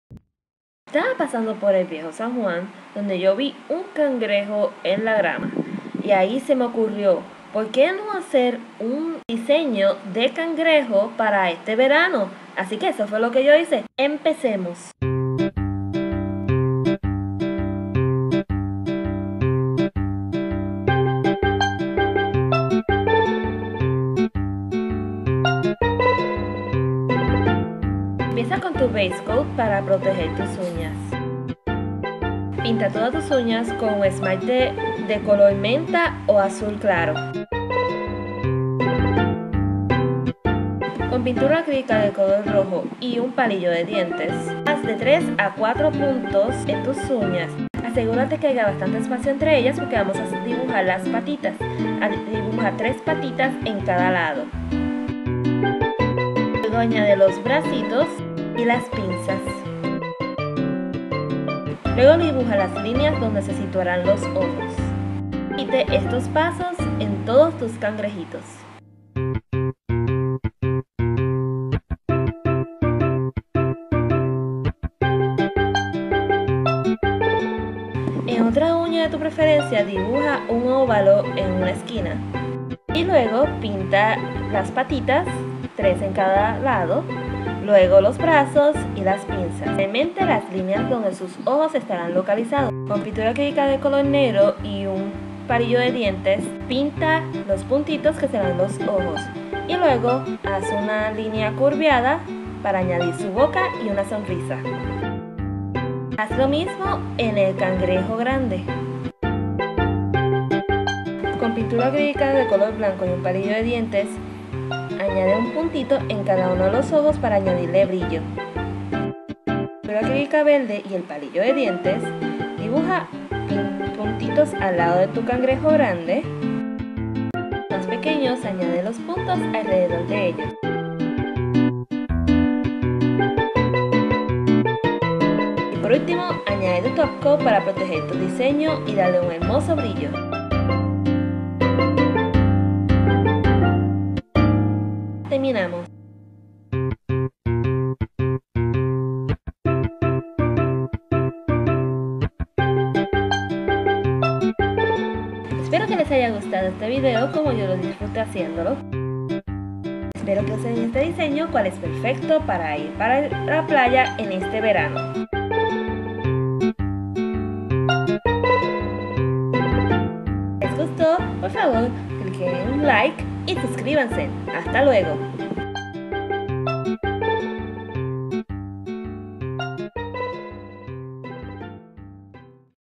CRAB NAIL ART TUTORIAL | Spanish with English subs!
Passing by Old San Juan (Yes, front of El Morro) I found a random crab walking around and it gave me an idea for this nail art tutorial! Who wouldn't love wearing cute crabs for summer?!